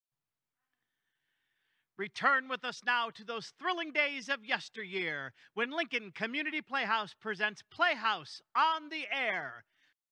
Welcome to Playhouse On The Air
Playhouse on the Air

Return with us now to those thrilling days of yesteryear when Lincoln Community Playhouse presents Playhouse on the Air, a series of old-time radio recreations. Each Friday at noon, the Playhouse will upload a new radio performance.

These audio performances are free.